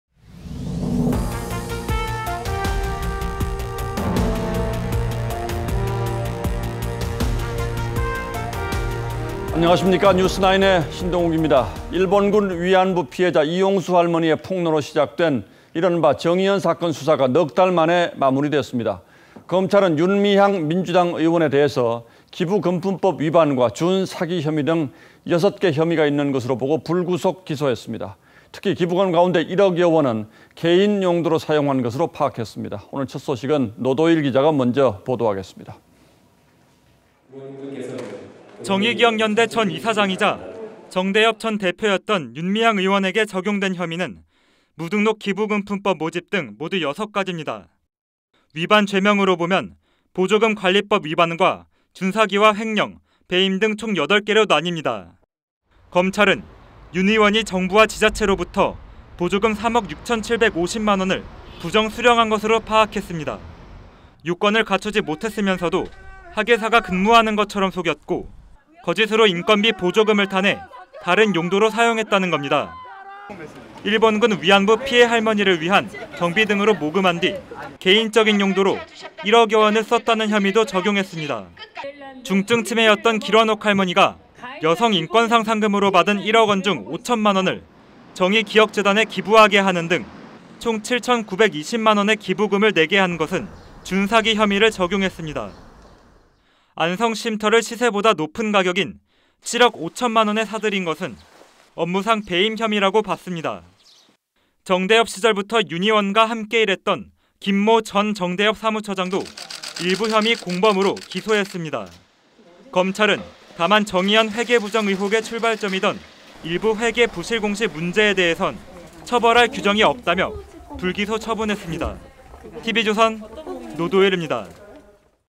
검찰, 사기·횡령 등 혐의 윤미향 기소…"개인 용도로 1억 썼다" [뉴스 9]
일본군 위안부 피해자 이용수 할머니의 폭로로 시작된 이른바 '정의연 사건'수사가 넉달만에 마무리됐습니다. 검찰은 윤미향 민주당 의원에 대해 기부금품법 위반과 준사기 혐의등 6개 혐의가 있는 것으로 보고 불구속 기소했습니다. 특히 기부금 가운데 1억여원은 개인 용도로 사용한 것으로 파악했습니다.